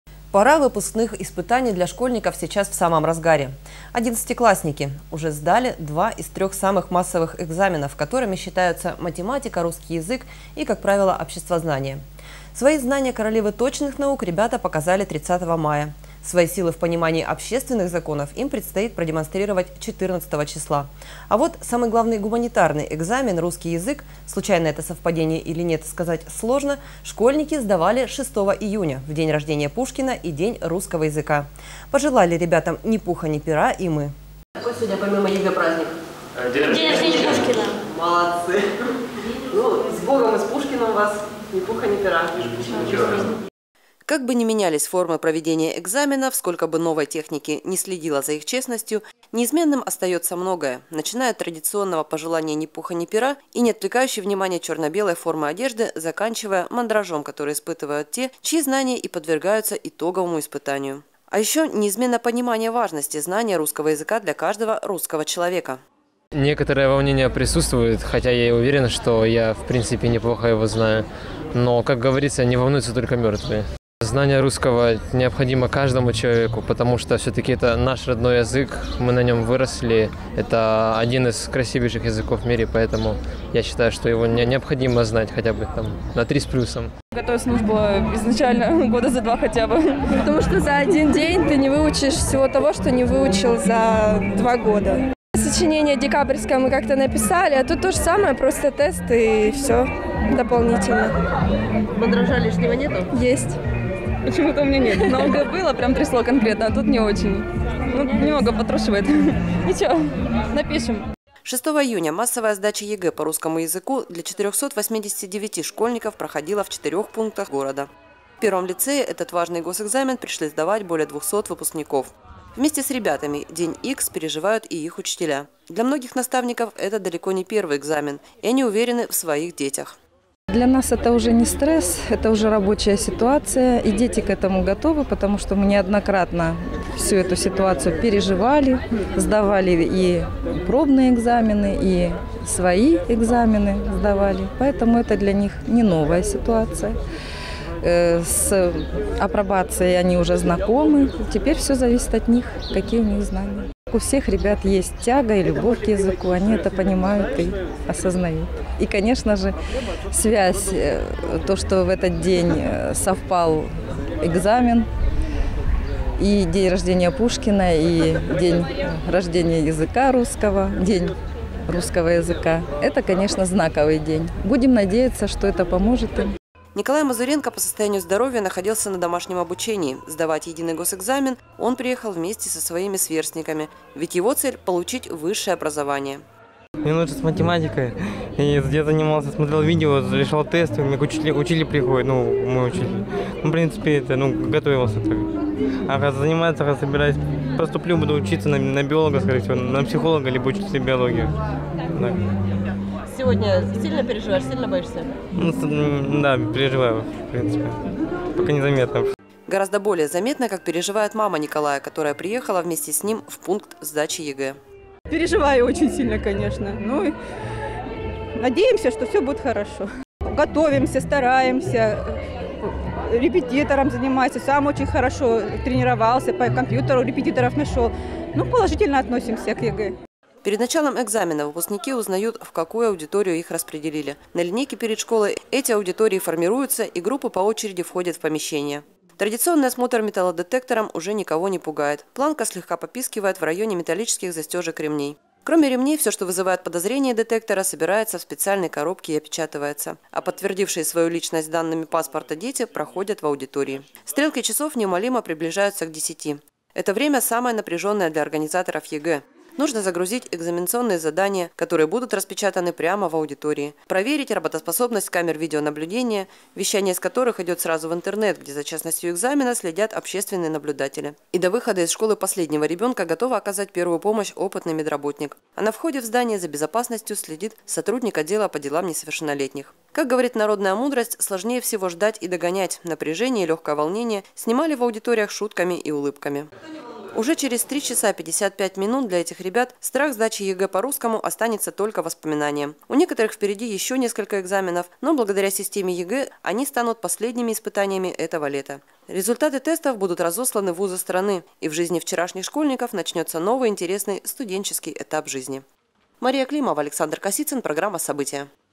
Массовая сдача ЕГЭ по русскому языку прошла для всех выпускников страны
Пора выпускных испытаний для школьников сейчас в самом разгаре. Одиннадцатиклассники уже сдали два из трех самых массовых экзаменов, которыми считаются математика, русский язык и, как правило, обществознание. Свои знания королевы точных наук ребята показывали 30 мая, свои силы в понимании общественных законов им предстоит продемонстрировать 14 числа. А вот самый главный гуманитарный экзамен - русский язык - (Случайно это совпадение, или нет, сказать сложно), школьники сдавали 6 июня в День рождения Пушкина и день русского языка. Пожелали ребятам ни пуха, ни пера и мы.